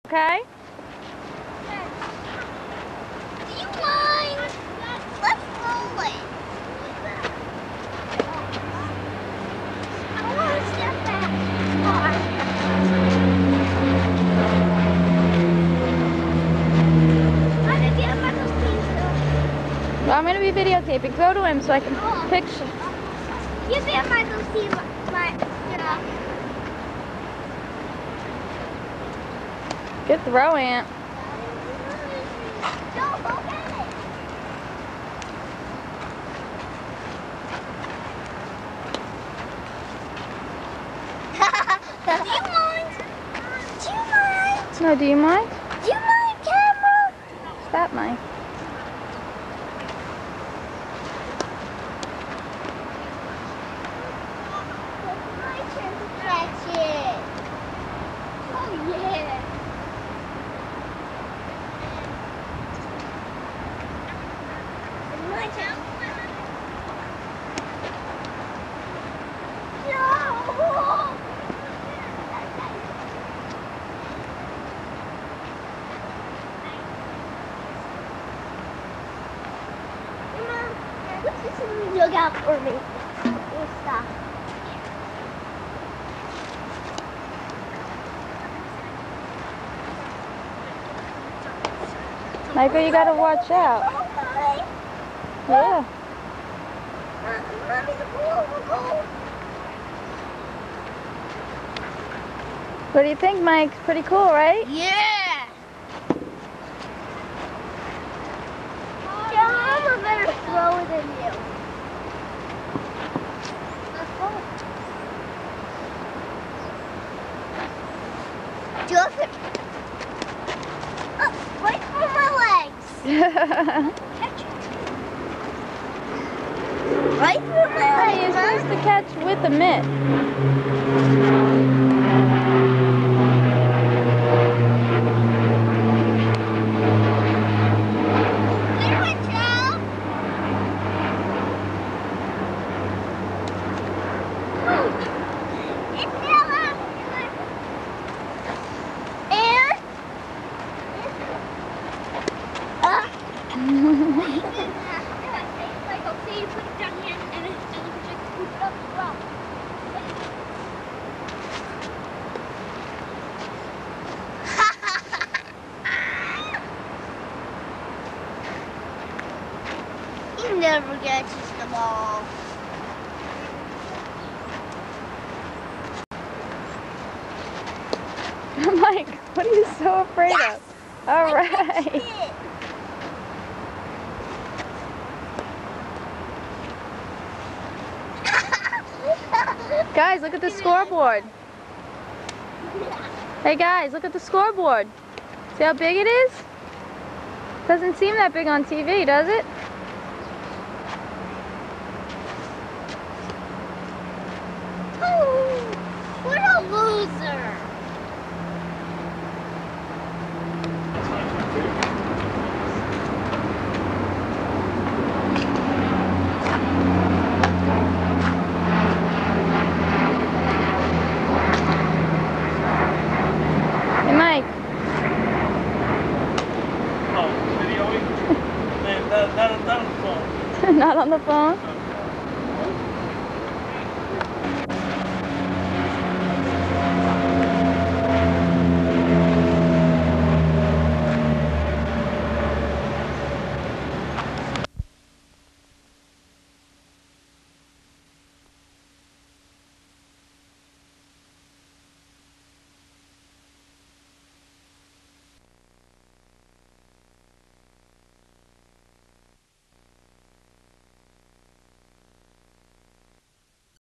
old shea stadium
having a catch at shea stadium 1995 ish